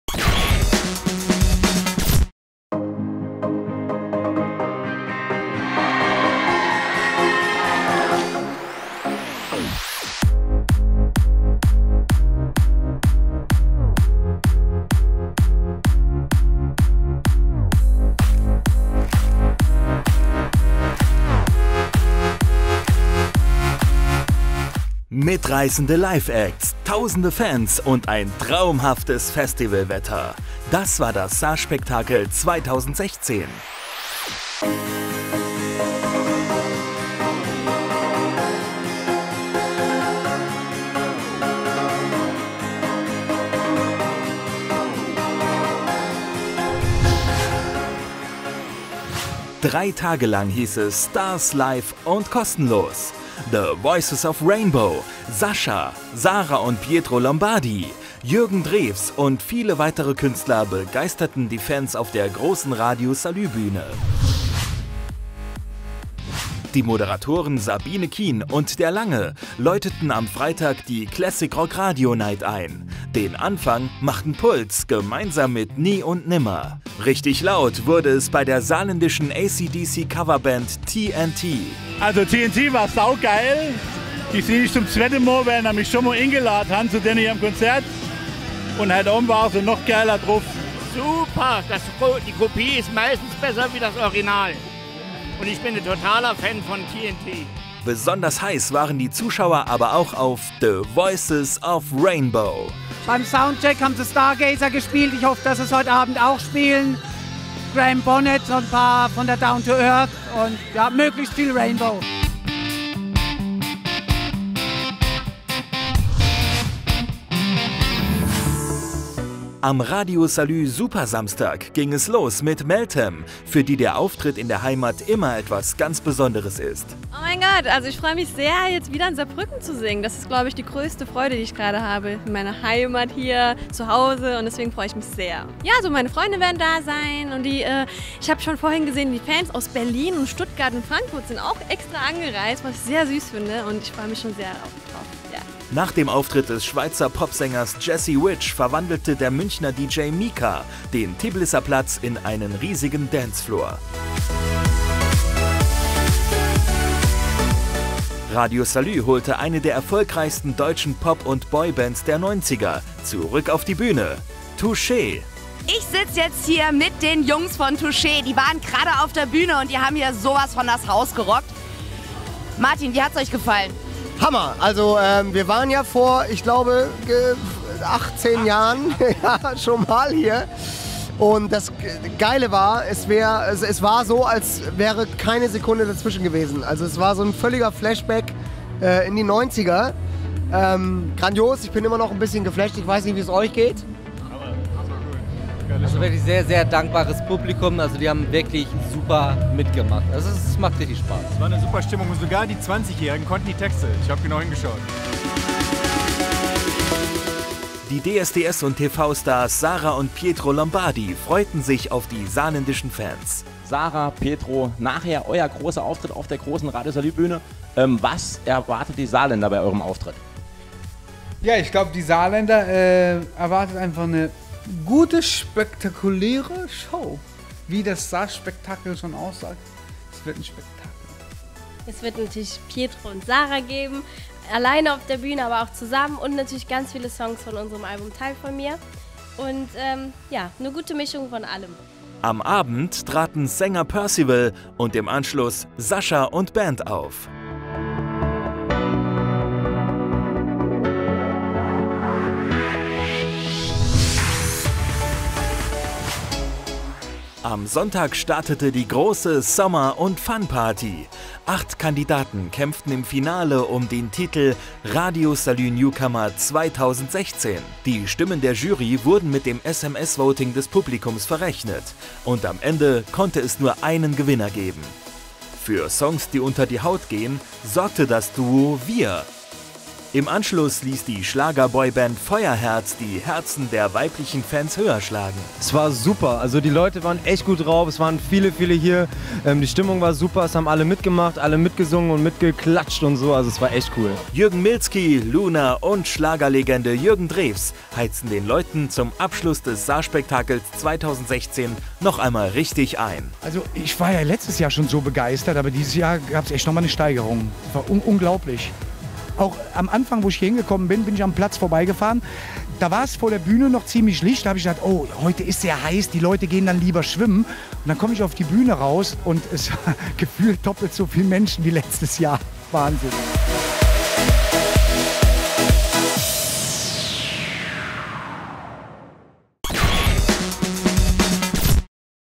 SAAR-SPEKTAKEL 2016 mit RADIO SALÜ
SASHA & Band, Sarah & Pietro Lombardi, Jürgen Drews und viele weitere regionale, nationale und internationale Stars traten beim SAAR-SPEKTAKEL 2016 auf!

Kamera/Schnitt: Kevin Berger